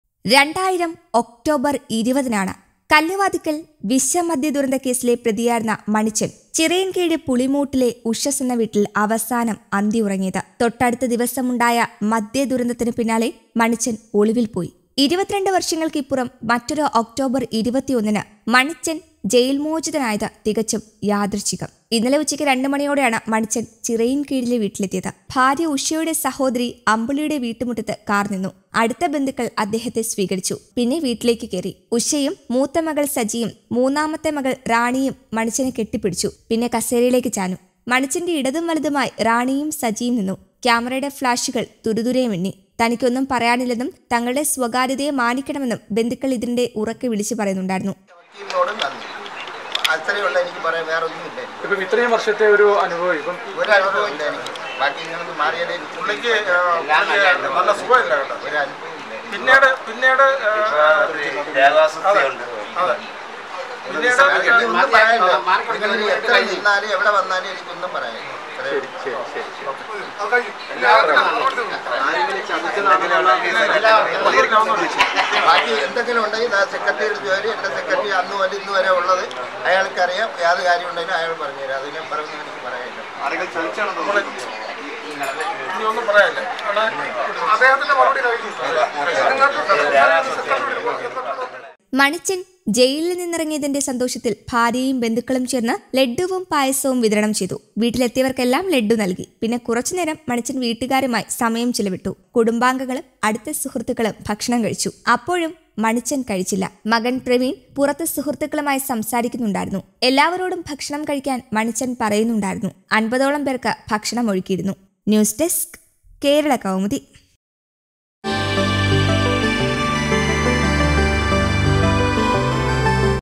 22 വര്‍ഷങ്ങള്‍ക്ക് ശേഷം മണിച്ചന്‍ ജന്മനാട്ടിലെത്തി..ഇഷ്ടവിഭവം വിളമ്പി ഭാര്യ. വാരിപുണര്‍ന്ന് മക്കള്‍
2000 ഒക്ടോബർ 20ന് ആണ് കല്ലുവാതുക്കൽ വിഷമദ്യ ദുരന്തകേസിലെ പ്രതിയായിരുന്ന മണിച്ചൻ ചിറയിൻകീഴ് പുളിമൂട്ടിലെ ഉഷസ് എന്ന വീട്ടിൽ അവസാനം  അന്തിയുറങ്ങിയത്. തൊട്ടടുത്ത ദിവസമുണ്ടായ മദ്യദുരന്തത്തിന് പിന്നാലെ മണിച്ചൻ ഒളിവിൽ പോയി. 22 വ‍ർഷങ്ങൾക്കിപ്പുറം മറ്രൊരു ഒക്ടോബർ 21ന് മണിച്ചൻ ജയിൽ മോചിതനായത് തികച്ചും യാദൃച്ഛികം. ഇന്നലെ ഉച്ചയ്‌ക്ക് രണ്ട് മണിയോടെയാണ് മണിച്ചൻ ചിറയിൻകീഴിലെ വീട്ടിലെത്തിയത്. എസ്.എൻ.ഡി.പി യൂണിയന്റെ കാർ മെയിൻ റോഡിൽ നിന്ന് മണിച്ചന്റെ വീട്ടിലേക്കുള്ള ഇടുങ്ങിയ വഴികടന്ന് മുന്നോട്ട് നീങ്ങുമ്പോൾ ഇടതുവശത്തുള്ള തന്റെ വീട്ടിലേക്ക് മണിച്ചൻ ഒന്നുനോക്കി. ഓർമ്മകൾ 22 കൊല്ലം പിന്നിലേക്ക് പാഞ്ഞു. സെക്കൻഡുകൾക്കുള്ളിൽ ഭാര്യ ഉഷയുടെ സഹോദരി അമ്പിളിയുടെ വീട്ടുമുറ്റത്ത് കാർ നിന്നു. അടുത്ത ബന്ധുക്കൾ അദ്ദേഹത്തെ സ്വീകരിച്ചു. അതിനിടെ ചാനൽ കാമറന്മാർ മണിച്ചനെ വളഞ്ഞു. ഒരുനിമിഷൻ നിശബ്ദനായി അവർക്ക് മുന്നിൽ മണിച്ചൻ നിന്നു. പിന്നെ വീട്ടിലേക്ക് കയറി. ഉഷയും മൂത്ത മകൾ സജിയും മൂന്നാമത്തെ മകൾ റാണിയും മണിച്ചനെ കെട്ടിപ്പിടിച്ചു. വീട്ടുകാരുടെ സ്നേഹവായ്‌പിൽ അലിഞ്ഞ മണിച്ചൻ പല്ലില്ലാത്ത കീഴ്‌മോണ കാട്ടി ചിരിച്ചു. പിന്നെ കസേരയിലേക്ക് ചാ‍ഞ്ഞു. മണിച്ചെ ഇടത്തും വലത്തുമായി റാണിയും സജിയും നിന്നു. കാമറയുടെ ഫ്ളാഷുകൾ തുരുതുരെ മിന്നി. തനിക്കൊന്നും പറയാനില്ലെന്നും തങ്ങളുടെ സ്വകാര്യതയെ മാനിക്കണമെന്നും ബന്ധുക്കൾ ഇതിനിടെ ഉറക്കെ വിളിച്ചുപറയുന്നുണ്ടായിരുന്നു.

മാദ്ധ്യമപ്രവർത്തകരുടെ അഭ്യർത്ഥന മാനിച്ച് മണിച്ചൻ പ്രതികരണത്തിന് തയ്യാറായി. വൻവെളിപ്പെടുത്തൽ പ്രതീക്ഷിച്ച് ചാനൽ കാമറകളും മൈക്കുകളും മണിച്ചന് മുമ്പിൽ നിരന്നു. എനിക്ക് ഒന്നും പറയാനില്ല. ഒരു അനുഭവവും ഇല്ല. ഇവിടെയല്ല എവിടെ വന്നാലും എനിക്ക് ഒന്നും പറയാനില്ല. കൂടുതൽ എന്തെങ്കിലും അറിയണമെങ്കിൽ എസ്.എൻ.ഡി.പി യൂണിയൻ സെക്രട്ടറിയോട് ചോദിക്കണം. അന്നുമുതൽ ഇന്നുവരെയുള്ള കാര്യങ്ങൾ അദ്ദേഹത്തിനറിയാം. അതിനപ്പുറം ഒന്നും പറയാനില്ലെന്ന് ആവർത്തിച്ച് ചോദ്യങ്ങളിൽ നിന്ന് മണിച്ചൻ ഒഴിഞ്ഞുമാറി.

മണിച്ചൻ ജയിലിൽ നിന്നിറങ്ങിയതിന്റെ സന്തോഷത്തിൽ ഭാര്യയും ബന്ധുക്കളും ചേർന്ന് ലഡ്ഡുവും പായസവും വിതരണം ചെയ്തു. വീട്ടിലെത്തിയവർക്കെല്ലാം ലഡ്ഡു നൽകി. പിന്നെ കുറച്ചുനേരം മണിച്ചൻ വീട്ടുകാരുമായി സമയം ചെലവിട്ടു. ഇതിനിടെ സിറ്റൗട്ടിൽ നിന്ന് അകത്തേക്ക് മണിച്ചൻ കടന്ന് ഇരുന്നു. അവിടെ മക്കളുമായി സ്വകാര്യ സംഭാഷണം. ഇടയ്ക്ക് വീട്ടിലേക്ക് കടന്നുവന്ന മാദ്ധ്യമപ്രവർത്തകരോട് ഒന്നും പറയാനില്ലെന്ന് മണിച്ചൻ ആവർത്തിച്ചു. ഏതാണ്ട് 45 മിനിട്ട് കഴിഞ്ഞതോടെ ഭക്ഷണം കഴിക്കാനുള്ള തയ്യാറെടുപ്പായി. മണിച്ചൻ എത്തുന്നതിനാൽ ഫ്രൈഡ് റൈസും ചിക്കനുമൊക്കെ ഏർപ്പാട് ചെയ്തിരുന്നു. കുടുംബാംഗങ്ങളും അടുത്ത സുഹൃത്തുക്കളും ഭക്ഷണം കഴിച്ചു. അപ്പോഴും മണിച്ചൻ കഴിച്ചില്ല. മകൻ പ്രവീൺ പുറത്ത് സുഹൃത്തുക്കളുമായി സംസാരിക്കുന്നുണ്ടായിരുന്നു. എല്ലാവരോടും ഭക്ഷണം കഴിക്കാൻ മണിച്ചൻ പറയുന്നുണ്ടായിരുന്നു. അമ്പതോളം പേർക്ക് ഭക്ഷണം ഒരുക്കിയിരുന്നു.

 ജീവകാരുണ്യ പ്രവർത്തനം തുടരും
മണിച്ചൻ മുമ്പും ജീവകാരുണ്യ പ്രവർത്തനം നടത്തിയിരുന്നെന്നും അത് ഇനിയും തുടരുമെന്നും എസ്.എൻ.ഡി.പി ചിറയിൻകീഴ് താലൂക്ക് യൂണിയൻ സെക്രട്ടറി ശ്രീകുമാർ പെരുങ്ങുഴി പറഞ്ഞു. ദൈവത്തിന്റെയും സാധാരണക്കാരുടെയും പ്രാർത്ഥന കൊണ്ടാണ്  മണിച്ചൻ മോചിതനായത്. ചിറയിൻകീഴിലെ സാധാരണക്കാർക്ക് മണിച്ചനെ കുറിച്ചുള്ള കാഴ്ചപ്പാട് വേറെയാണ്. മരുന്നായിട്ടും ദിവസേനയുള്ള അന്നത്തിനായിട്ടും യൂണിയൻ വഴി,​ മണിച്ചൻ സഹായങ്ങൾ ചെയ്തിട്ടുണ്ട്. ഇനിയും അത് തുടരാനുള്ള ആഗ്രഹം മണിച്ചൻ അറിയിച്ചതായും ശ്രീകുമാർ വെളിപ്പെടുത്തി. ചിറയിൻകീഴ് താലൂക്ക് യൂണിയൻ വൈസ് പ്രസി‌ഡന്റ് പ്രദീപ് സഭവിള,​ എസ്.എൻ ട്രസ്റ്റ് മെമ്പർ ബിജു തോന്നയ്ക്കൽ,​ പുതുക്കരി ശാഖാ സെക്രട്ടറി സുരേഷ് തിട്ടയിൽ എന്നിവരും മണിച്ചനൊപ്പമുണ്ടായിരുന്നു.